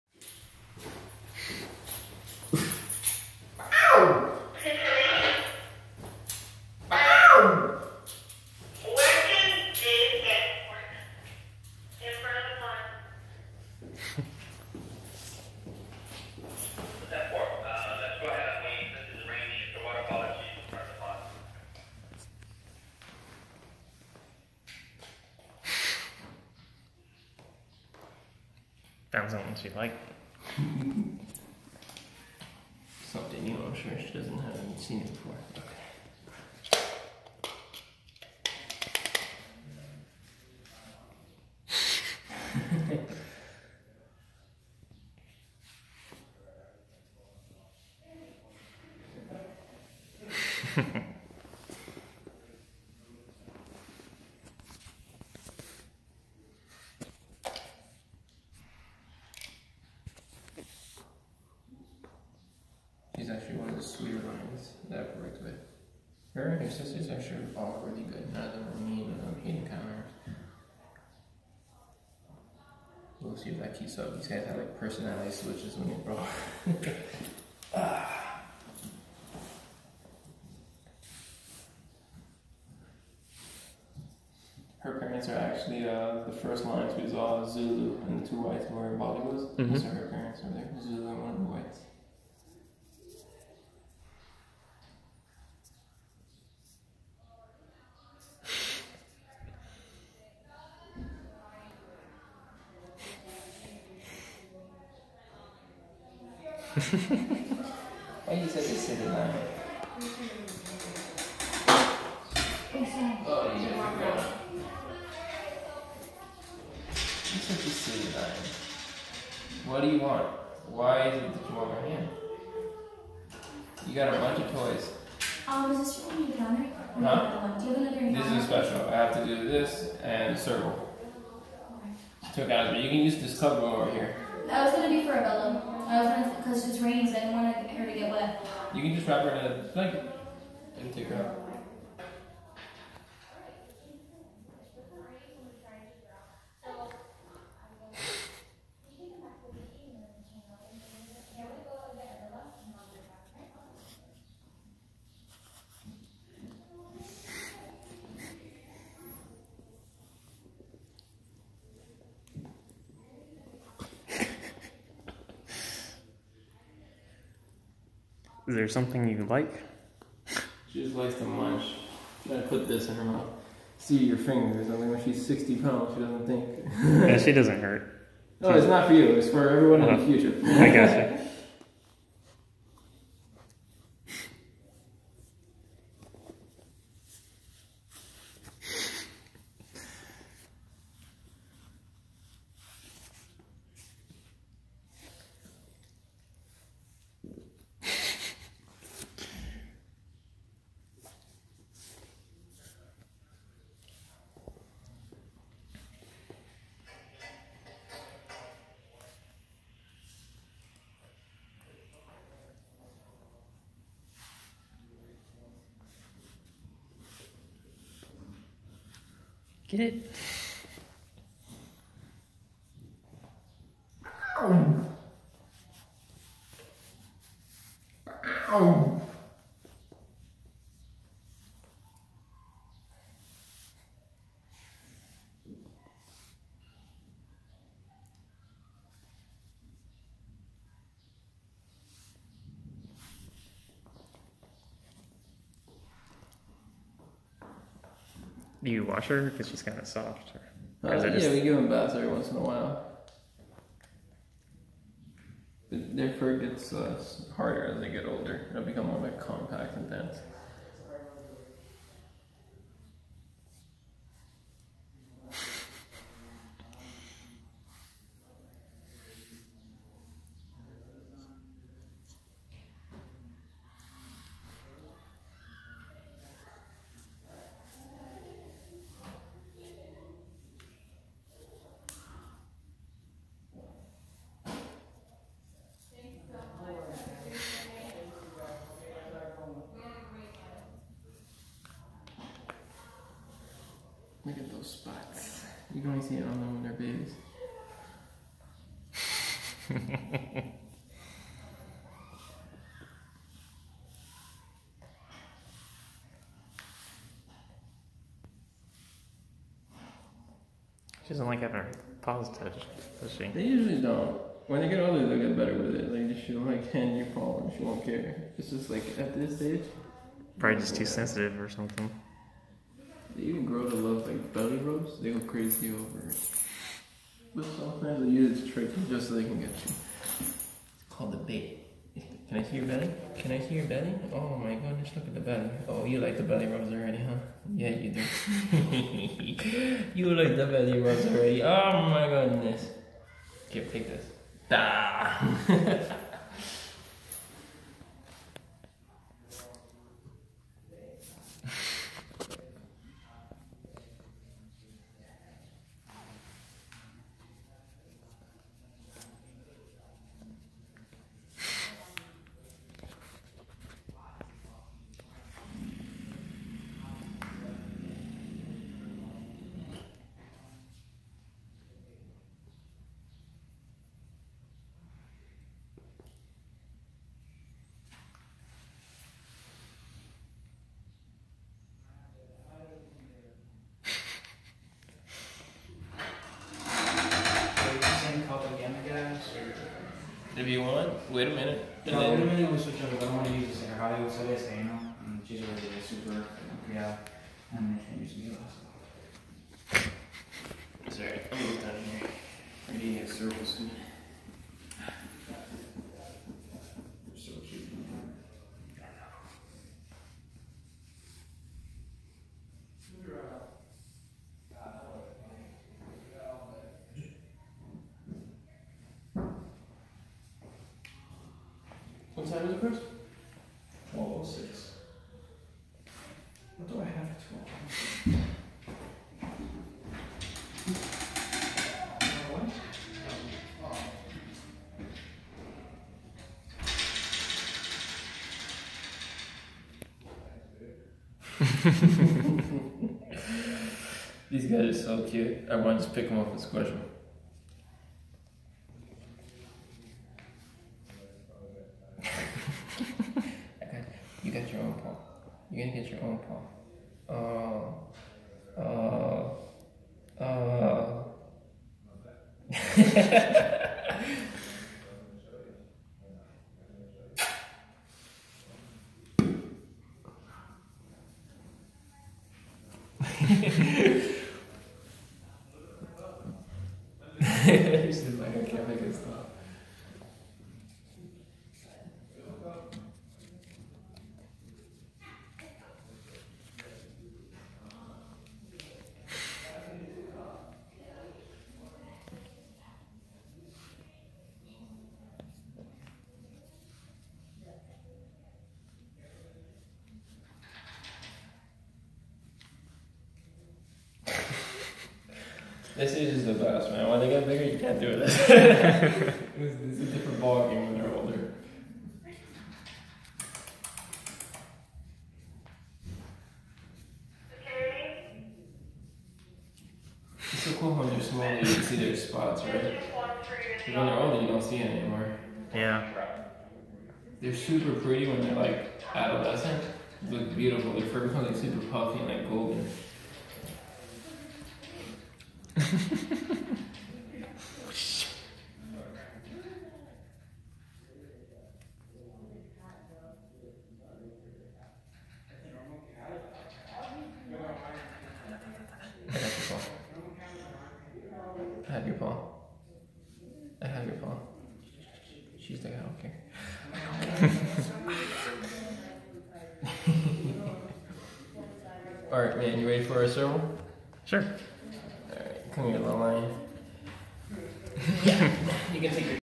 Playing with a White Lion Cub
Playing with a White Lion Cub at the Zoological Wildlife Foundation in Miami, FL.